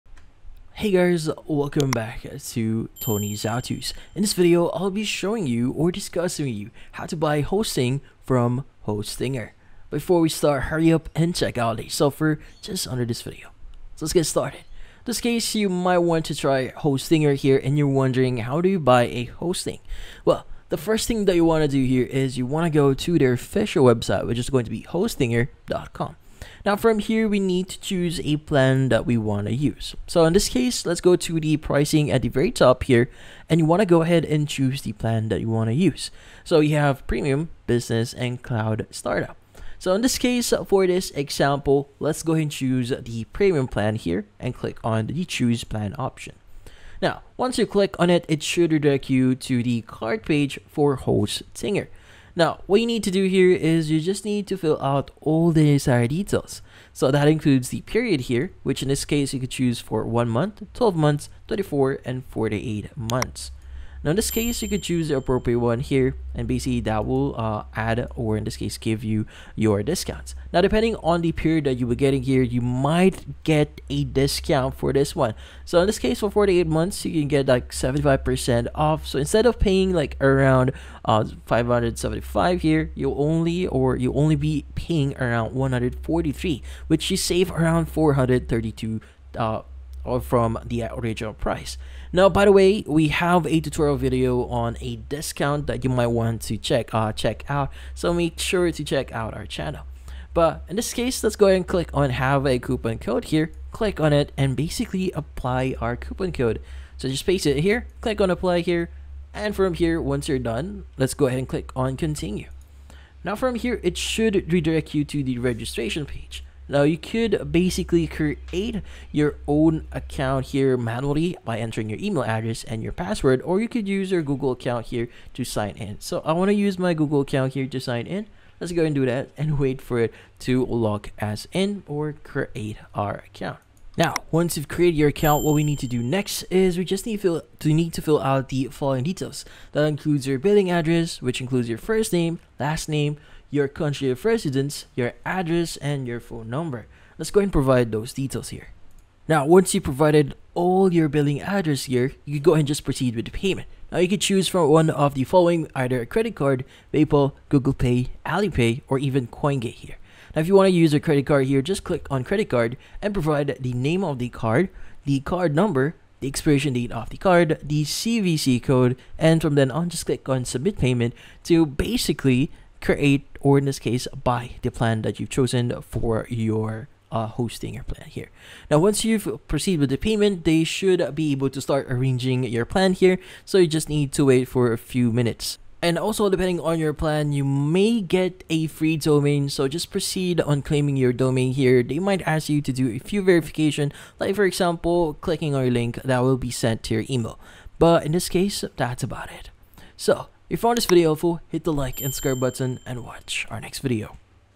How to Buy Hosting From Hostinger | Free Domain Offered (2024)
In this video, I show you How to Buy Hosting From Hostinger | Free Domain Offered in 2024. The quick and easy tutorial how to buy hostinger hosting with free domain. Do you want to know step by step how to get good hosting in 2024? Yes? Great!